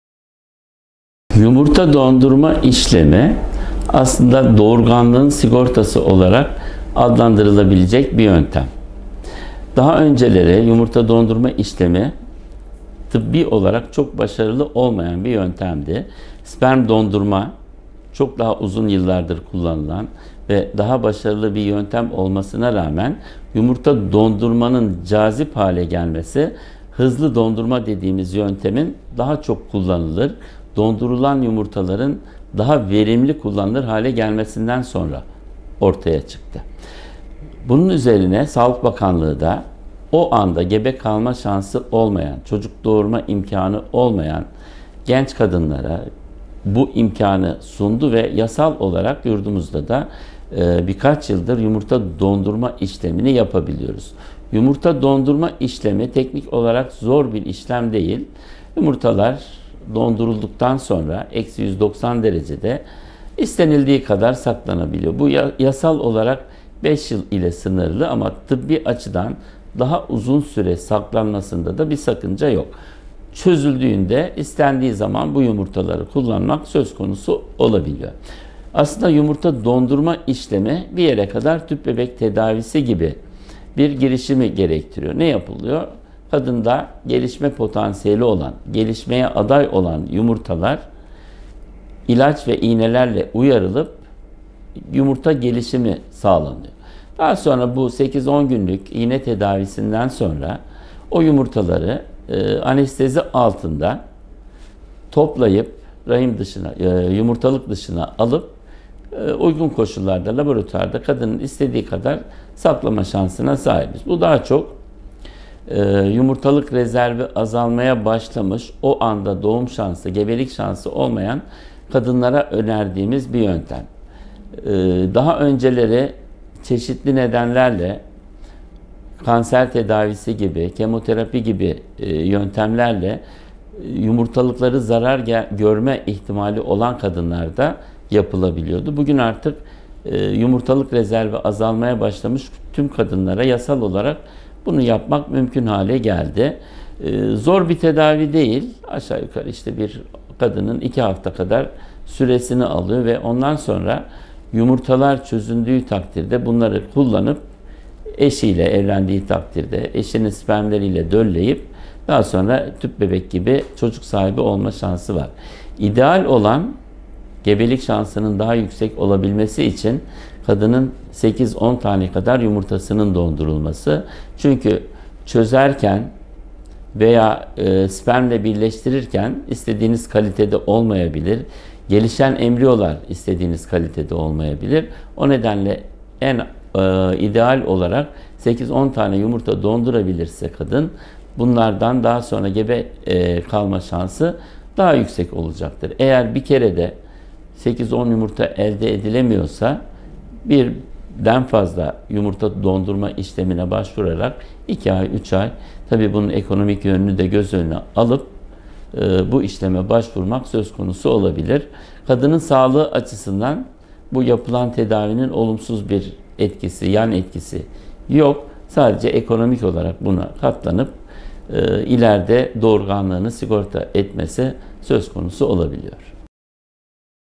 Yumurta dondurma işlemi nasıl yapılır? - Prof. Dr. Faruk Buyru
Prof. Dr. Faruk Buyru "Yumurta dondurma işlemi nasıl yapılır?" sorusunun cevabını veriyor.

 Yumurta dondurma işlemi aslında doğurganlığın sigortası olarak adlandırılabilecek bir yöntemdir. Daha önceleri yumurta dondurma işlemi tıbbi olarak çok başarılı olmayan bir yöntemdi. Sperm dondurma çok daha uzun yıllardır kullanılan ve çok başarılı bir yöntem olmasına rağmen yumurta dondurmanın cazip hale gelmesi hızlı dondurma dediğimiz yöntemin daha çok ve dondurulan yumurtaların daha verimli kullanılır hale gelmesinden sonra ortaya çıktı. Bunun üzerine Sağlık Bakanlığı'da o anda gebe kalma şansı olmayan, çocuk doğurma imkanı olmayan genç kadınlara bu imkanı sundu ve yasal olarak yurdumuzda da bir kaç yıldır yumurta dondurma işlemini yapabiliyoruz.
                    Yumurta dondurma işlemi teknik olarak çok zor bir işlem değil. Yumurtalar -190 derecede dondurulduktan sonra istenildiği kadar saklanabilir. Bu yasal olarak 5 yıl ile sınırlı ama tıbbi açıdan daha uzun süre saklanmasında bir sakınca yok. Çözüldüğünde istendiği zaman bu yumurtaları kullanmak söz konusu olabilir. Aslında yumurta dondurma işlemi bir yere kadar tüp bebek tedavisi gibi bir girişimi gerektiriyor. Kadında gelişme potansiyeli olan gelişmeye aday olan yumurtalar ilaç ve iğnelerle uyarılıp yumurta gelişimi sağlanıyor. Daha sonra bu 8-10 günlük iğne tedavisinden sonra o yumurtaları anestezi altında toplayıp yumurtalık dışına alıp uygun laboratuar koşullarında kadının istediği kadar saklama şansına sahibiz.
                     Bu daha çok yumurtalık rezervi azalmaya başlamış o anda gebelik şansı olmayan kadınlara önerdiğimiz bir yöntem. Daha önceleri kanser tedavisi kemoterapi gibi çeşitli nedenlerle yumurtalıkları zarar görme ihtimali olan kadınlarda yapılabiliyordu, bugün artık yumurtalık rezervleri azalmaya başlamış tüm kadınlarda yasal olarak bunu yapmak mümkün hale geldi. Zor bir tedavi değil kadının 2 hafta kadar bir süresini alıyor. Yumurtalar çözüldüğü takdirde,  eşiyle evlendiğinde eşinin spermleriyle bunları dölleyip tüp bebek gibi daha sonra çocuk sahibi olma şansı var.
                     İdeal olan gebelik şansının daha çok olmabilmesi için kadının 8-10 tane yumurtasının dondurulmasıdır çünkü çözerken ya da spermle birleştirirken istediğimiz kalitede olmayabilir, gelişen embriolar istediğimiz kalitede olmayabilir. O nedenle ideal olarak kadın  8-10 yumurta dondurabilirse bunlardan daha sonra gebe kalma şansı daha yüksek olacaktır. Eğer bir kerede 8-10 yumurta elde edilemiyorsa birden fazla yumurta dondurma işlemine başvurarak 2-3 ay, tabi bunun ekonomik boyutunuda göz önüne alıp bu işleme baş vurmak söz konusu olabilir. Kadının sağlığı açısından bu yapılan tedavinin olumsuz bir yan etkisi yok. Sadece ekonomik olarak buna katlanıp ileride doğurganlığını sigorta etmesi söz konusu olabiliyor.